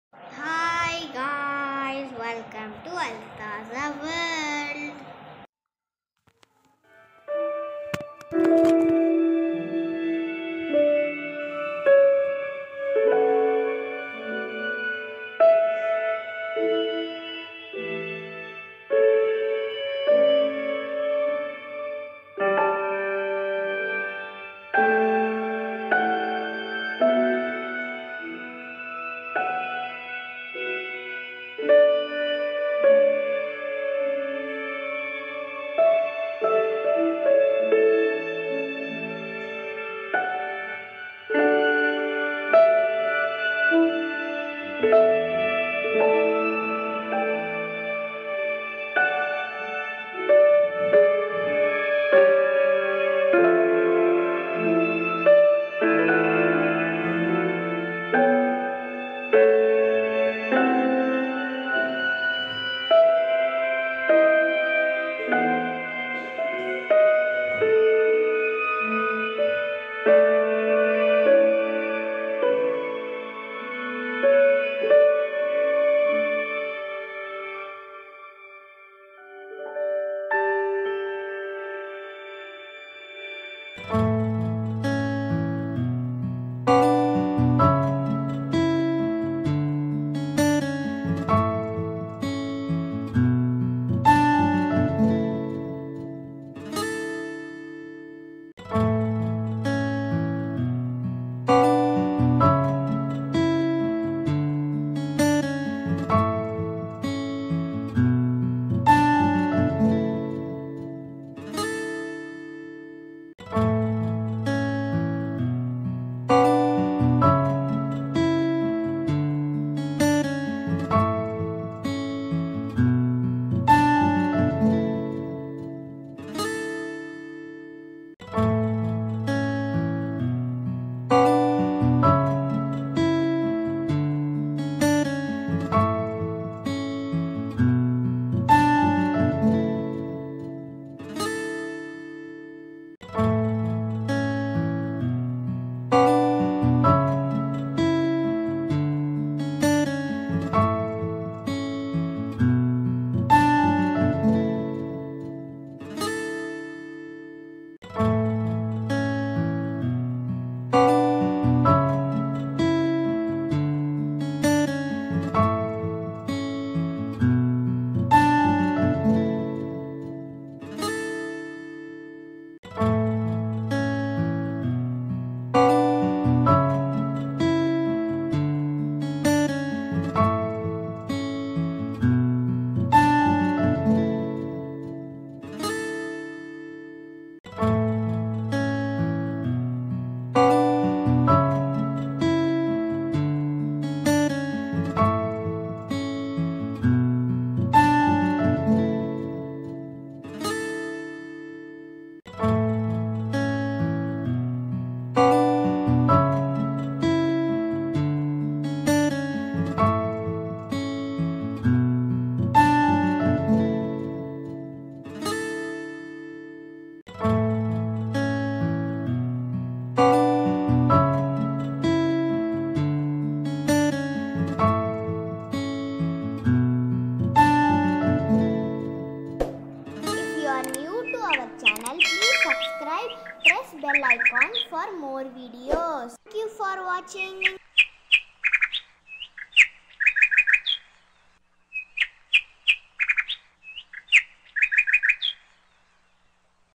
Birds nest from waste toy box || reuse idea|| ( craft ideas @tazaworld115 ).
craft ideas:
this video contains how to make birds nest from waste toy box, or reuse Idea from waste toys box .

(craft ideas @tazaworld115 ).